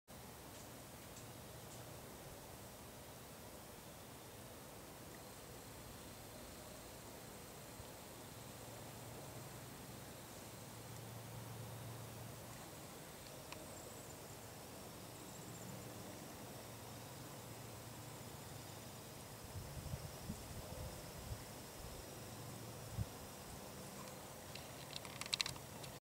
Wood Frog (Ranidae: Lithobates/Rana sylvatica) on Log
Photographed at Turtle River State Park, North Dakota (15 May 2010).